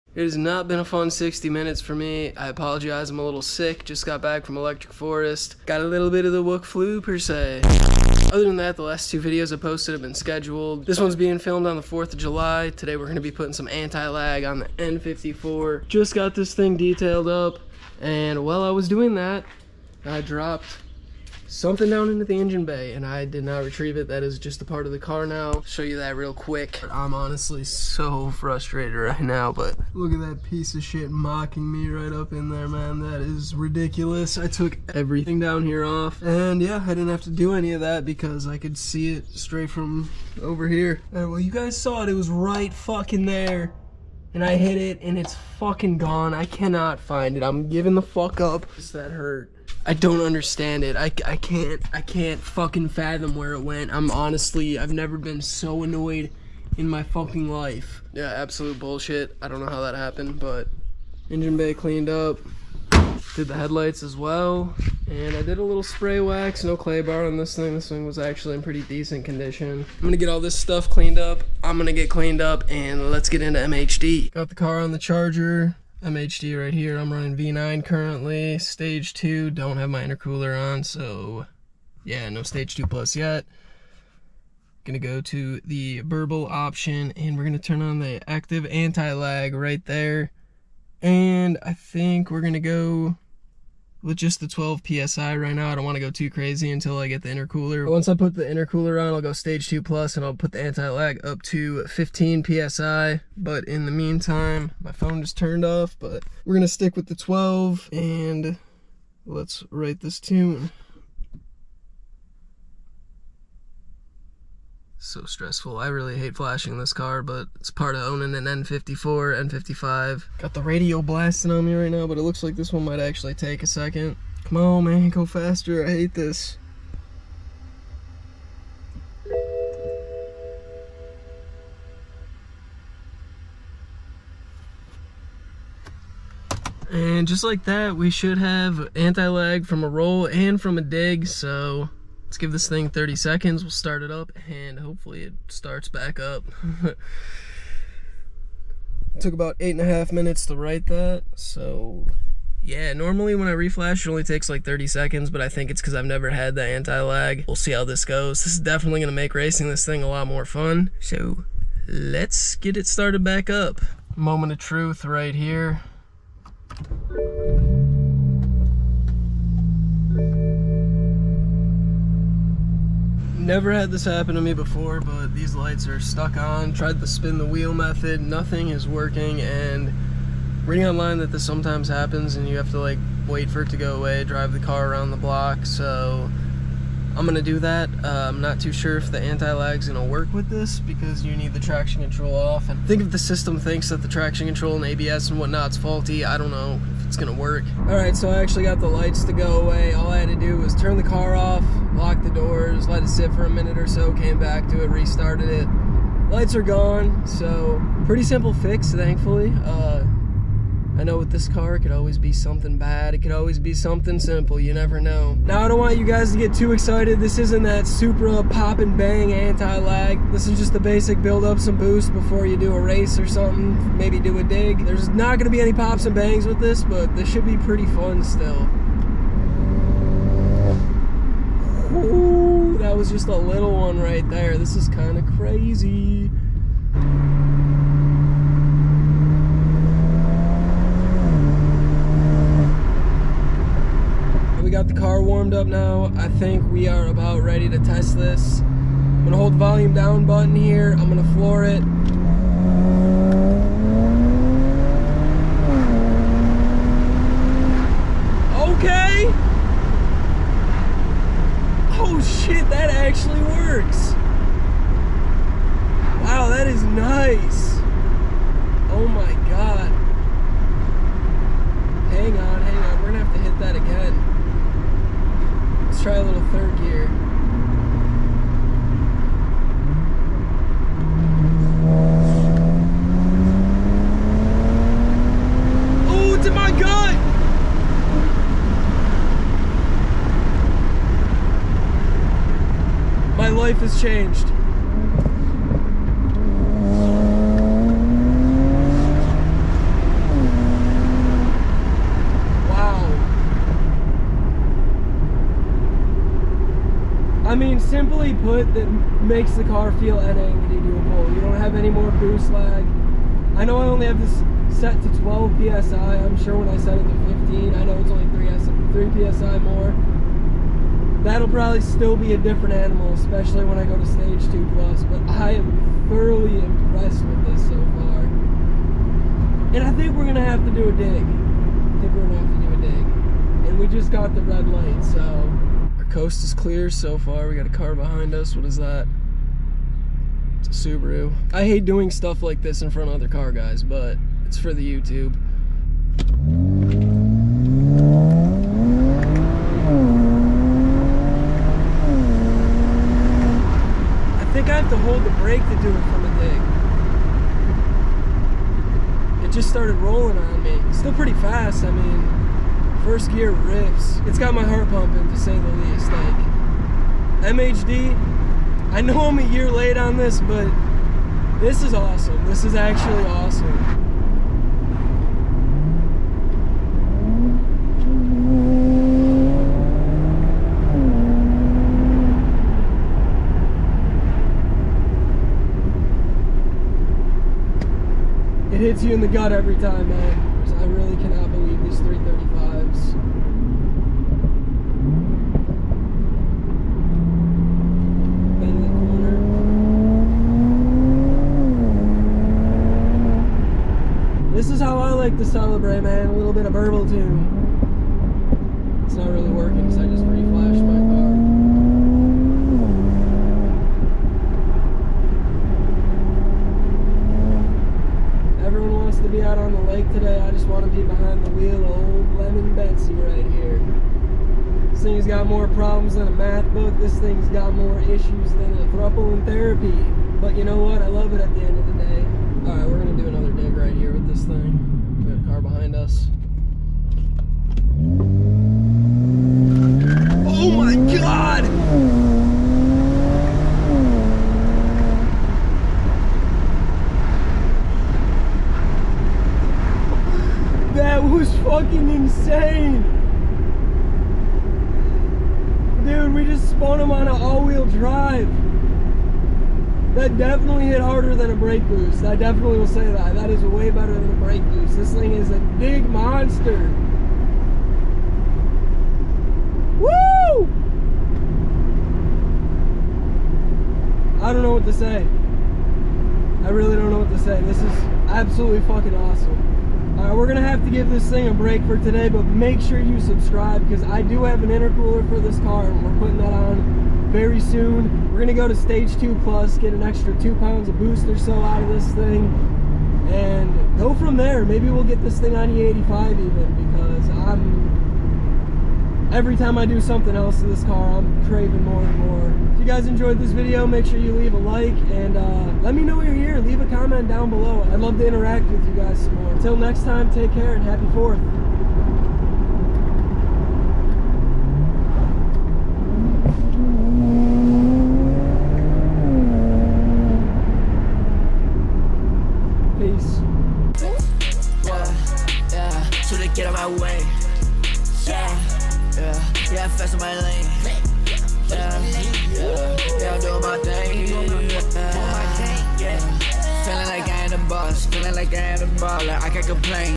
N54 BMW 335i GETS ANTILAG!!!
I finally got around to testing out the antilag feature on MHD which is still only currently available for the n54 335i to my knowledge. This feature is absolutely insane and makes the BMW a whole different beast, check out how it works in this video where i test it from both a roll and a dig! be sure to subscribe & leave a like if you enjoyed the video!

RANDOM TAGS